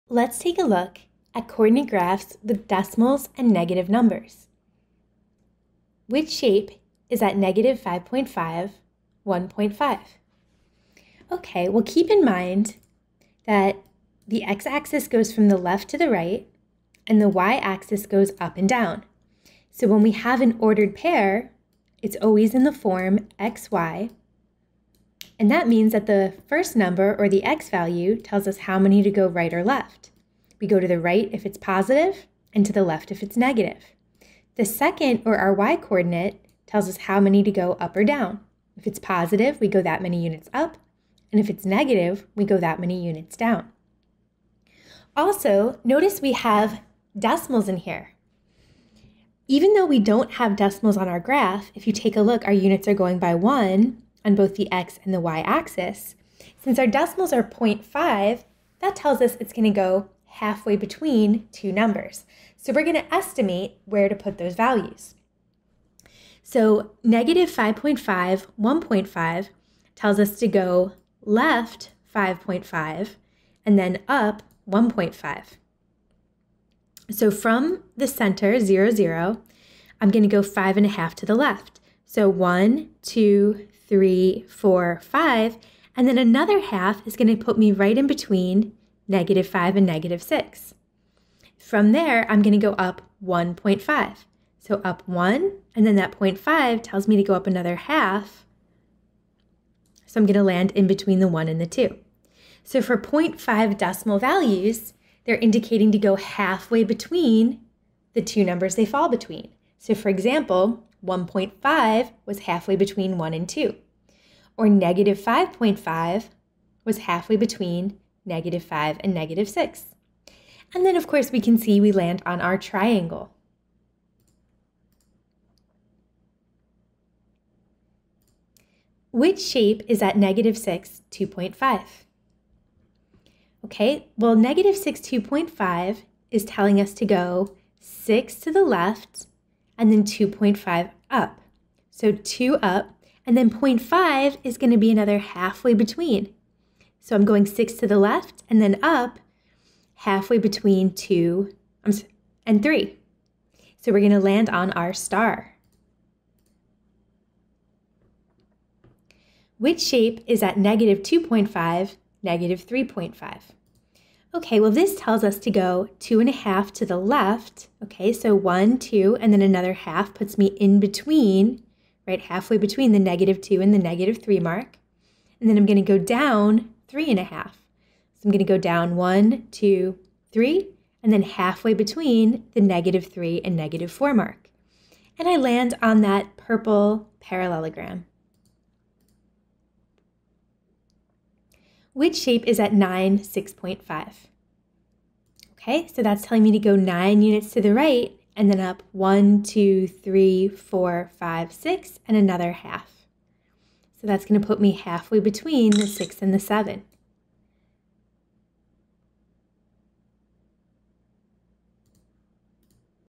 6.115 Coordinate Graphs with Decimals and Negative Numbers
Coordinate Graphs with Decimals and Negative Numbers
Practice finding an object at a given point on a coordinate plane.